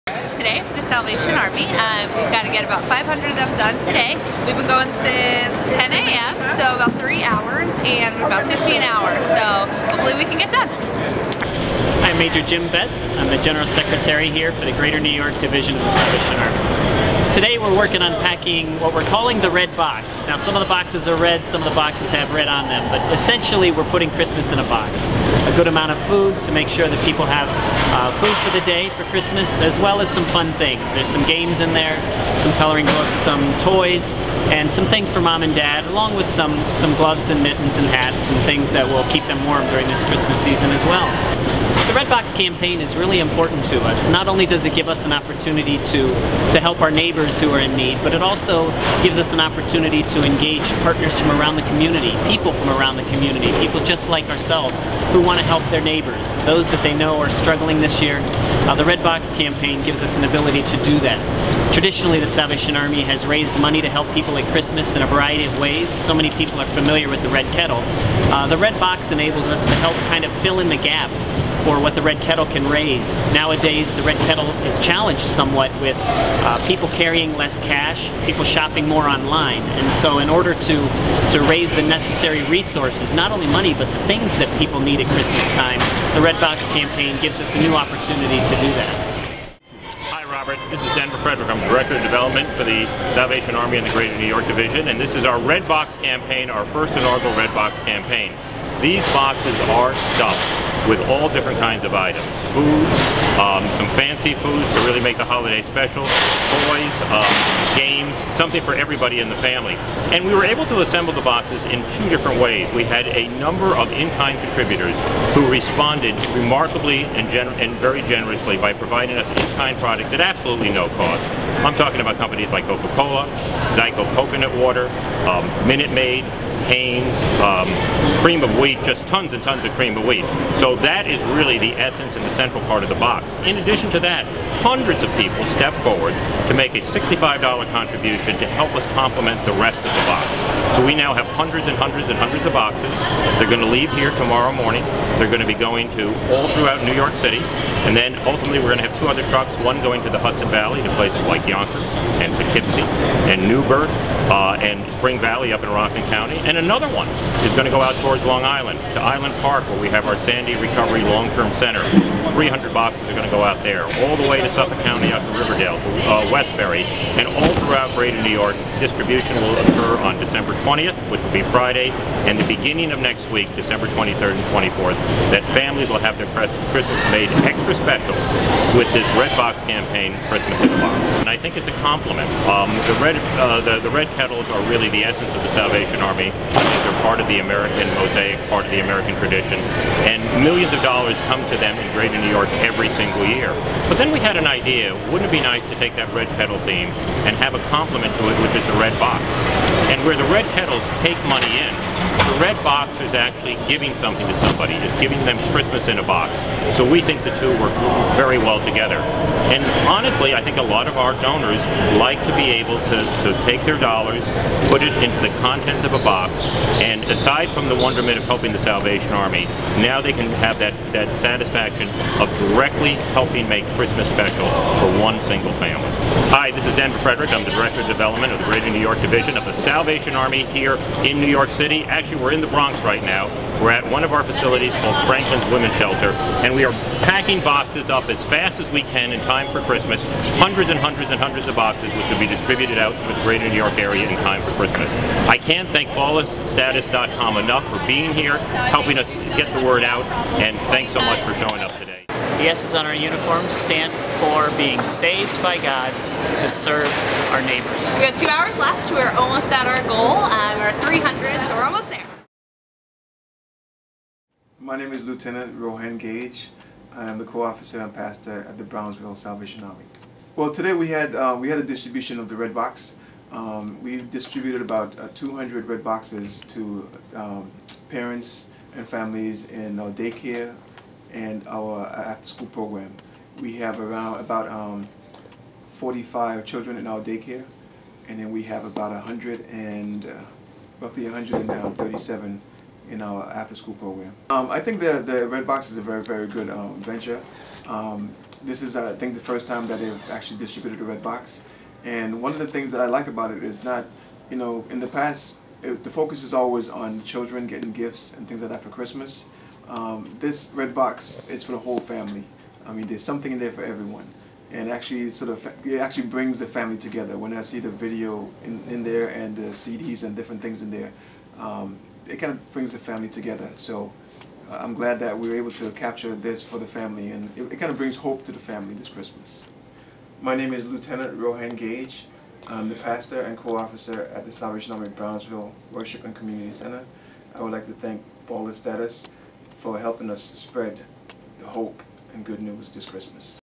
Salvation Army NYC Spreads Holiday Cheer Via Red Box Campaign
In the days leading up to Christmas, The Salvation Army Greater New York Division and volunteers worked together to help in the assembly and distribution of the Salvation Army Red Box Campaign, a program allowing individuals and companies to help bring "Christmas in a Box" to local families in need this holiday season.

We visited one of the Salvation Army's locations in the Bronx to talk to key officers about what the program was all about, as well as the assembly of these Red Boxes, filled with goodies for the whole family -- from companies such as Coca-Cola, Good Stuff Toys, BG Foods (Cream of Wheat & Pirates Booty), Minute Rice, White Rock Beverages, and Zico Coconut Water, among others.

Hope everyone have a happy holidays. For more info on the Salvation Army's Red Box Campaign,